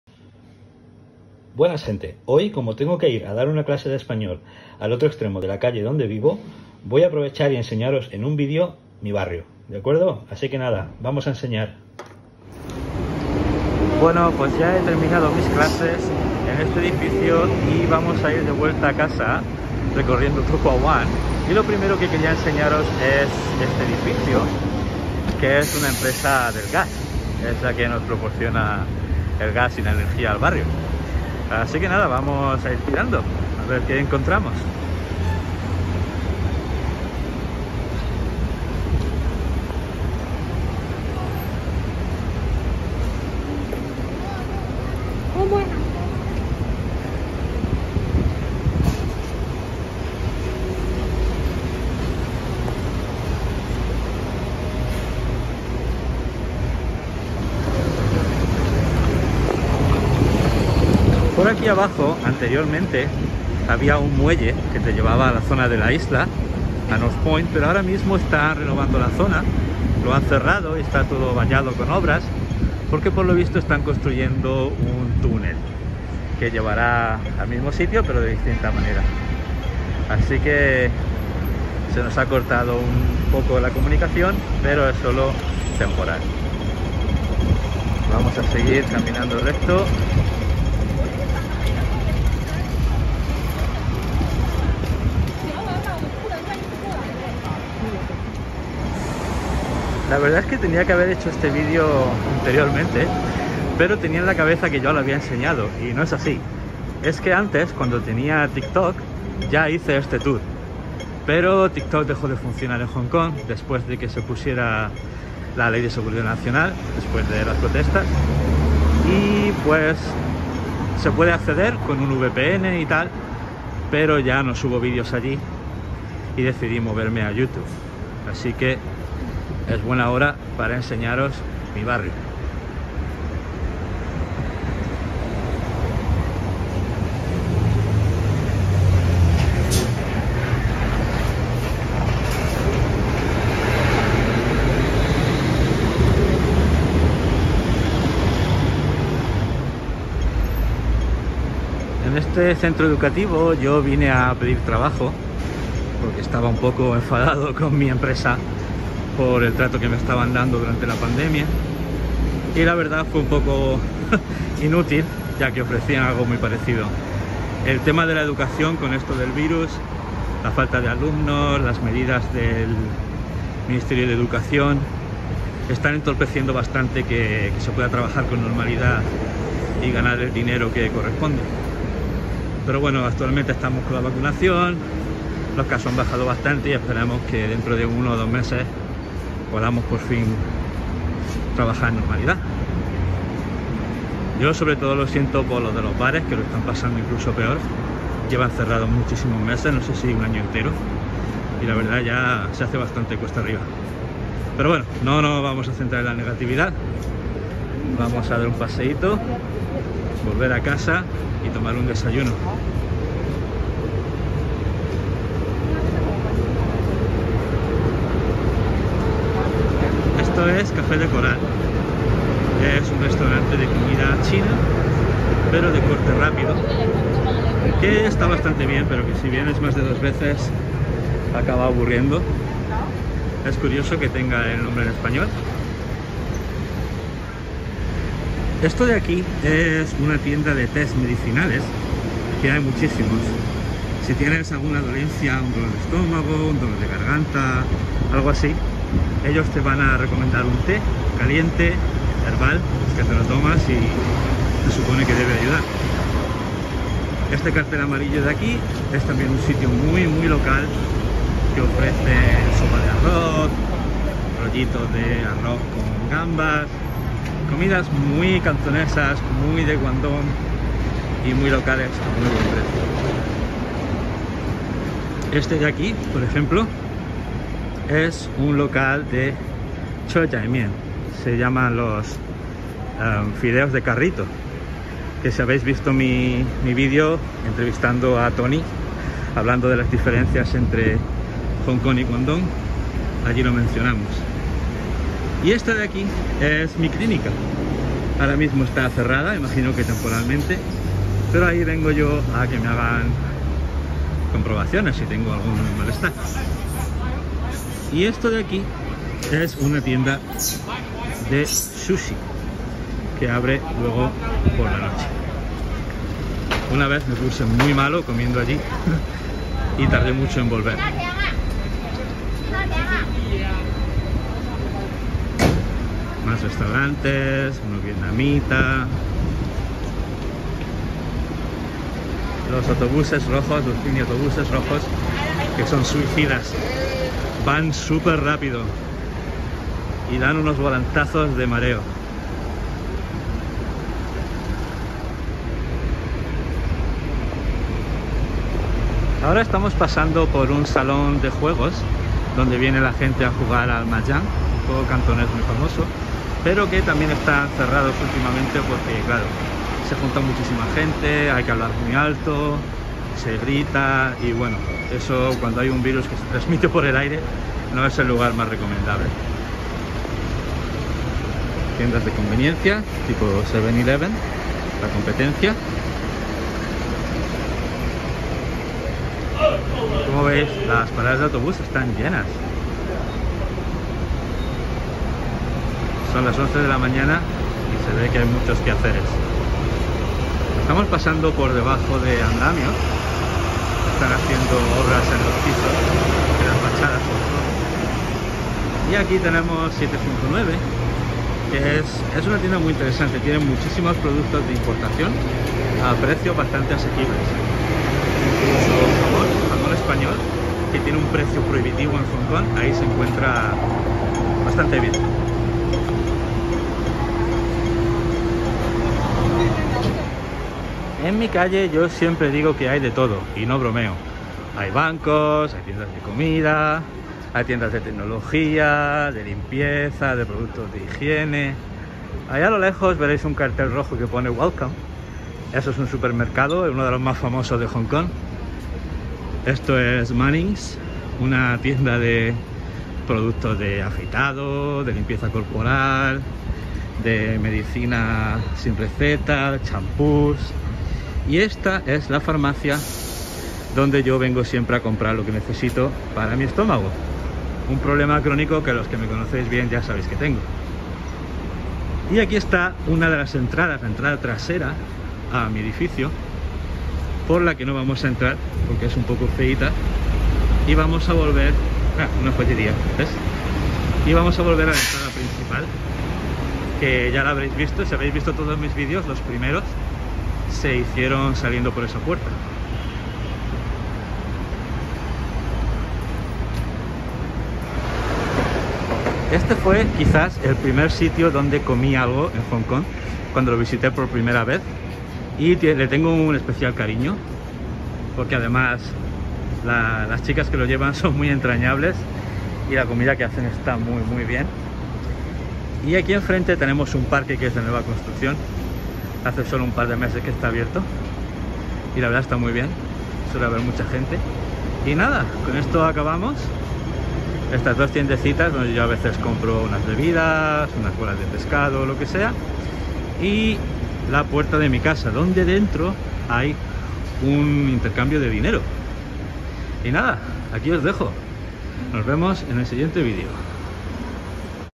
To Kwa Wan - 土瓜湾
Un paseo por mi barrio.